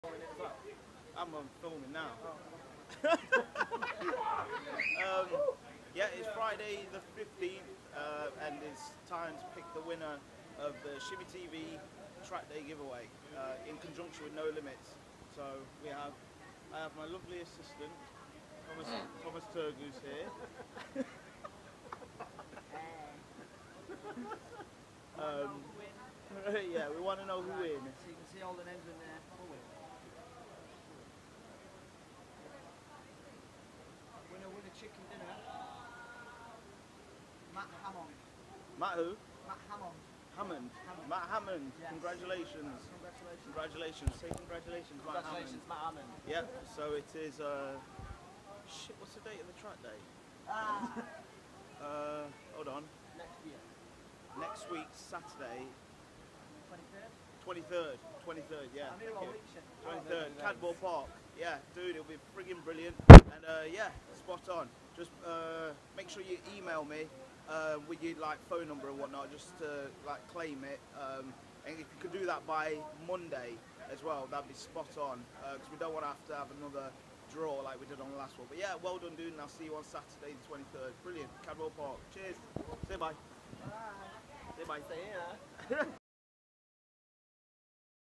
The winner of the @Shimmy_TV @NoLimitsOnTrack gizatrackday Trackday Giveaway is....
Today we're announcing the winner of the 2nd Shimmy TV Trackday Giveaway in conjunction with No Limits!!  Sorry about the pants video quality... Shimmy's mashed up his phone and isn't armed with the GoPros tonight!

Should you fail to get in touch we will have to find someone else to fill your place and that would be sad!

Huge thanks to all that entered and thank you for all your support!!! We'll be regularly doing giveaways and competitions so keep an eye out for the next!